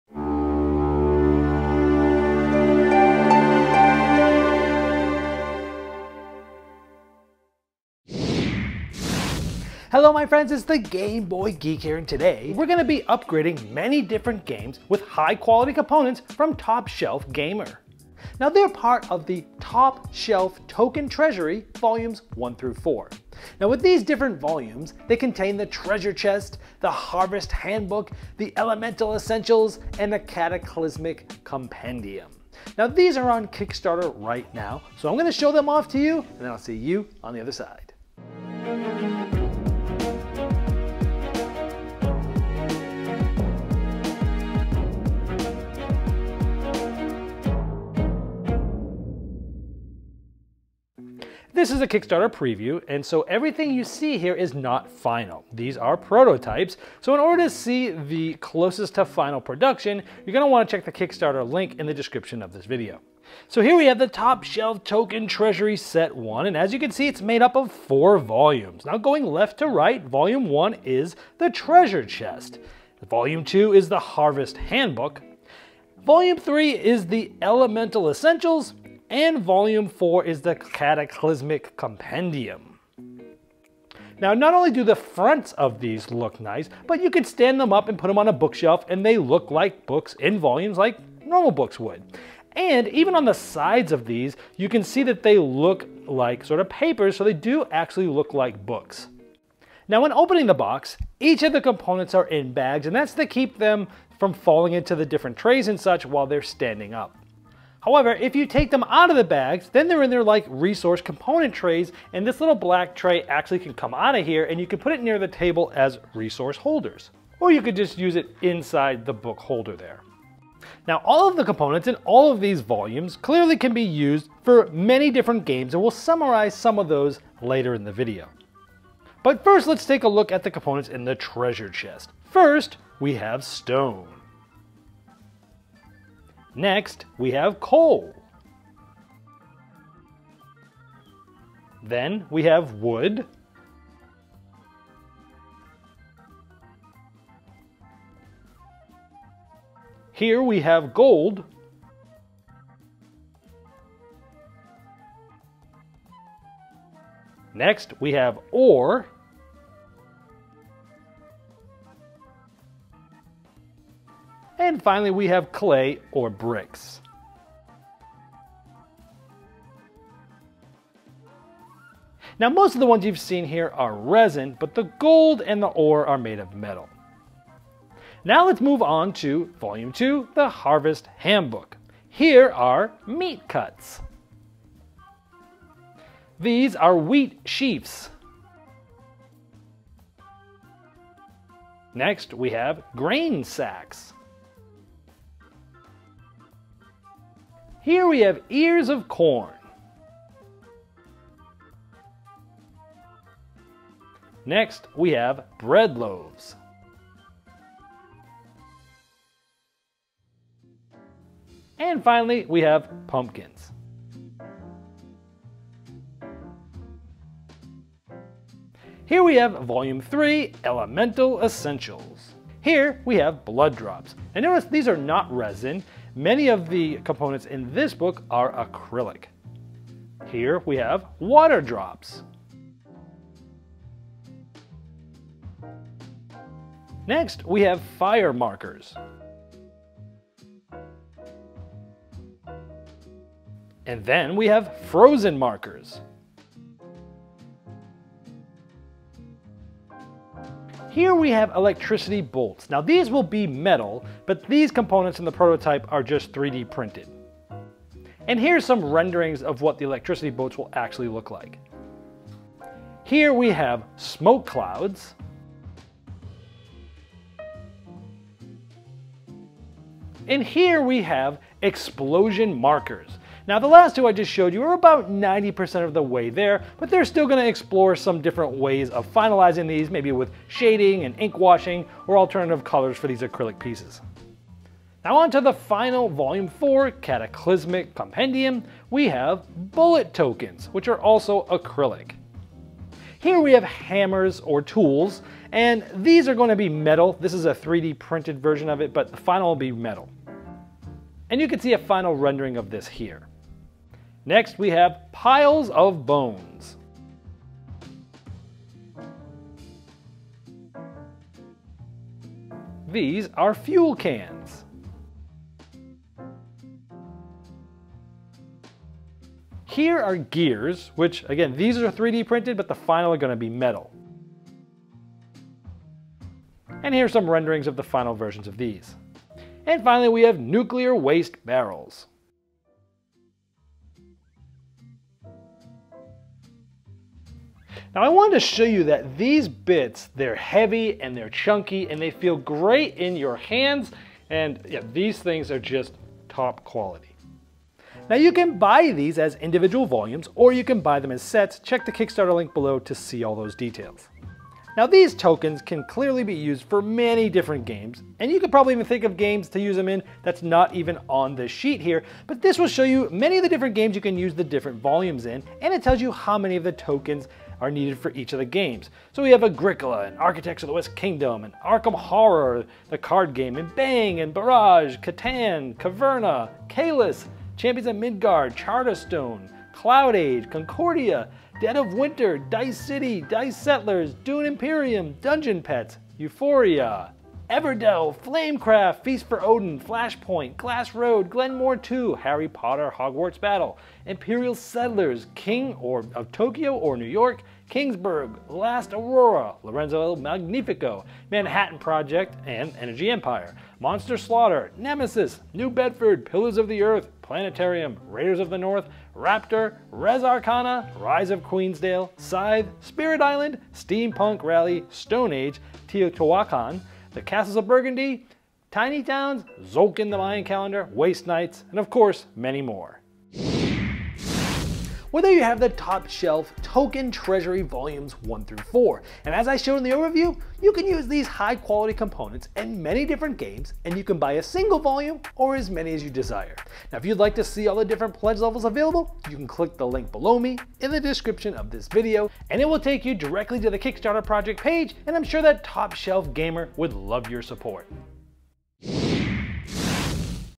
Top Shelf Token Treasury Preview with the Game Boy Geek
My review of the Top Shelf Gamer Token Treasury boxes that bring you high quality chunky bits to be used in many different games!